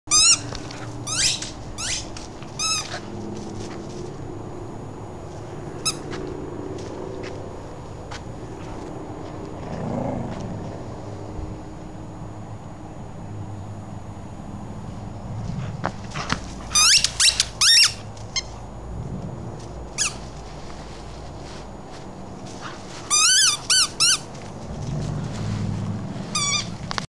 Bedlington terrier puppy and the ball.
Sqeeky clean.Nine months old liver coloured boy.